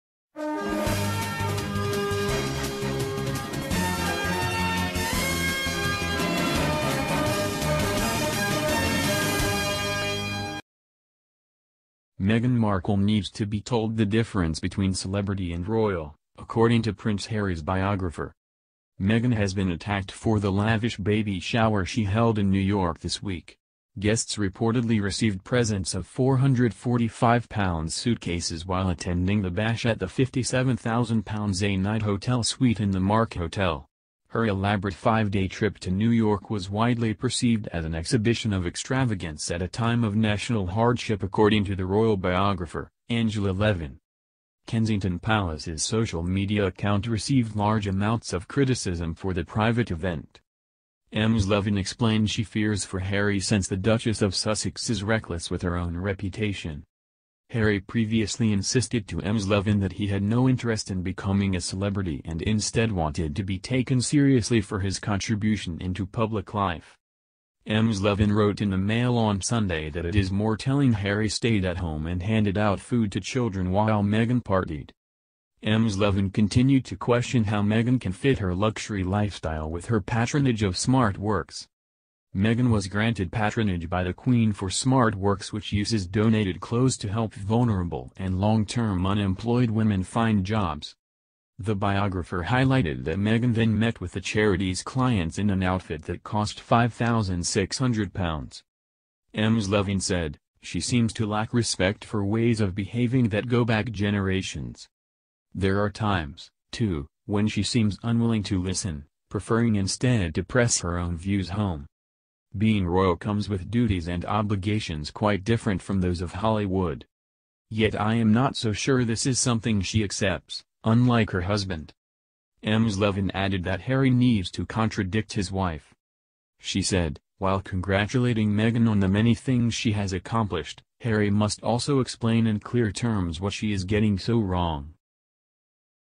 Meghan Markle is getting it SO wrong and Harry should warn her says biographer
Prince Harry should tell Meghan Markle ‘what she is doing SO wrong’ says royal biographer